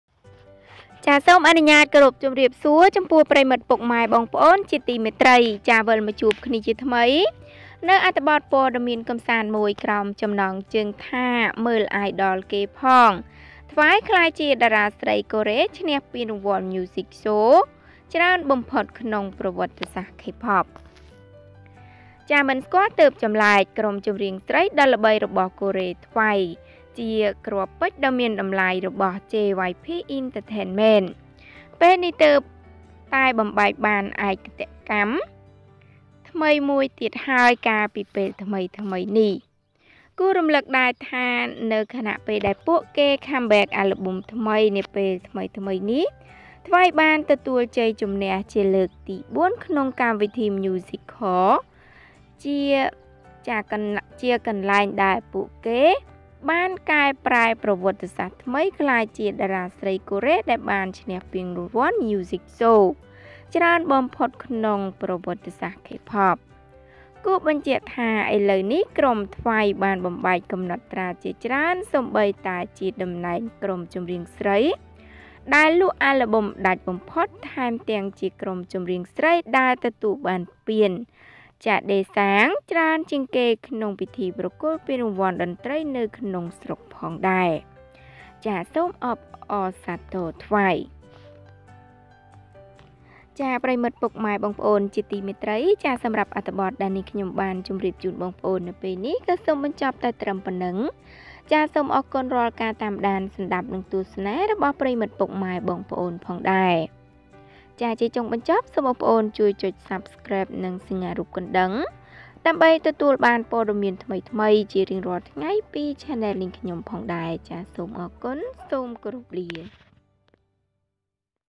មើល Idol គេផង! Twice ក្លាយជាតារាស្រីកូរ៉េ ឈ្នះពានរង្វាន់ Music Show ច្រើនបំផុត ក្នុងប្រវត្តិសាស្រ្ត
See also Idol!  Twice became the most popular Korean actress in Kpop history

សូមស្វាគមន៍មកកាន់បណ្តាញព័ត៌មានសូមកុំភ្លេចចុះឈ្មោះជាវប៉ុស្តិ៍របស់ខ្ញុំសូមអរគុណ!

សូមអរគុណយ៉ាងជ្រាវជ្រៅវ
Thank You So much !